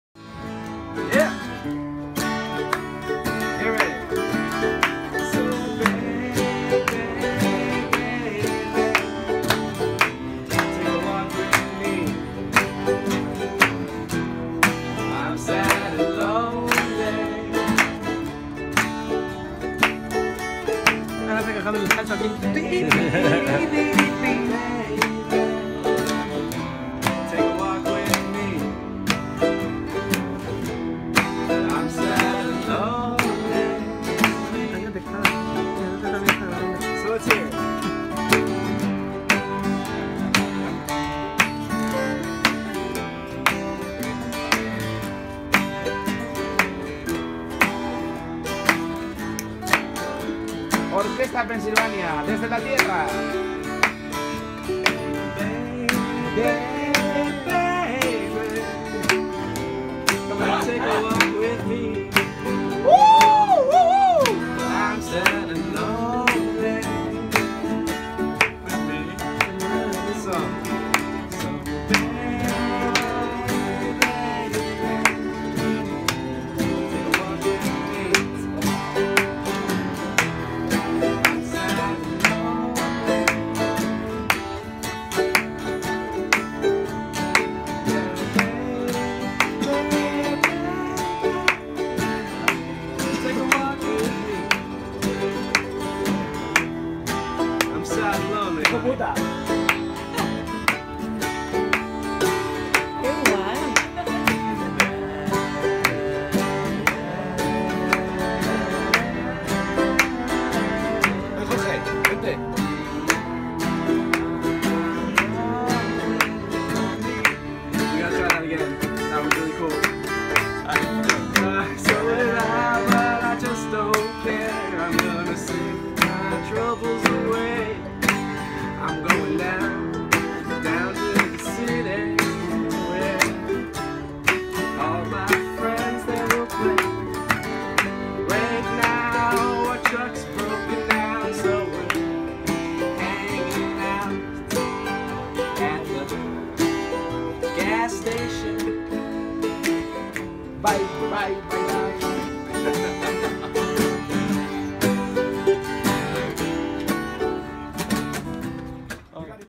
Igor Inza y Rubén Improvisando en acústico junto a músicos Americanos en carretera 2016
Igor Inza y Rubén improvisan en acústico con unos músicos americanos que encuentran durante su Gira 2016 con Orquesta Pensilvania. Experiencia inolvidable...